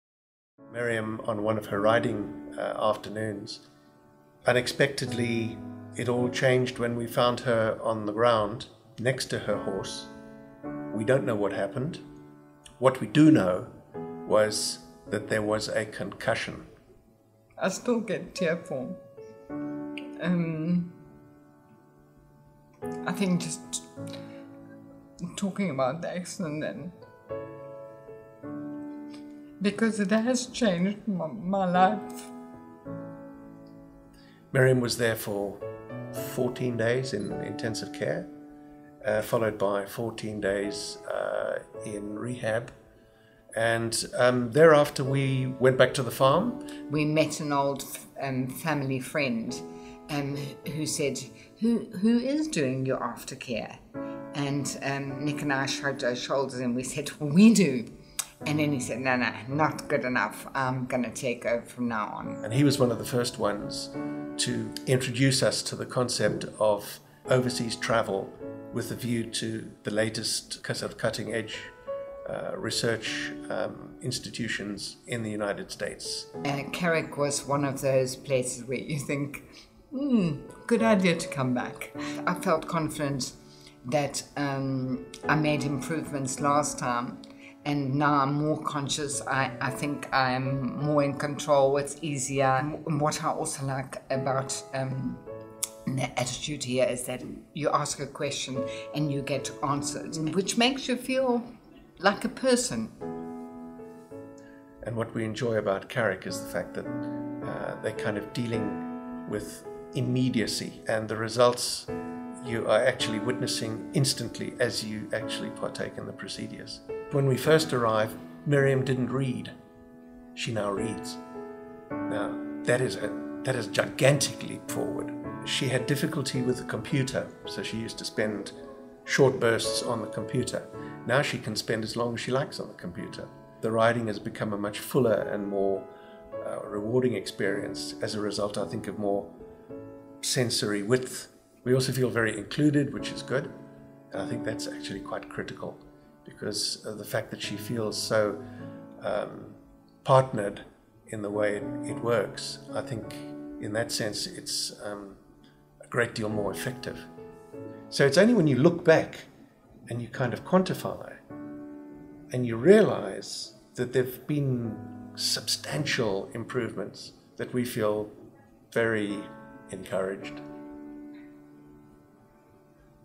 Concussion Treatment - Learning to read agin - Cerebrum Brain Treatment
Miriam fell off her horse while riding and suffered a concussion. After spending 4 weeks in the hospital, she was released but she still could not read or write. After visiting Cerebrum twice she can now read and write again.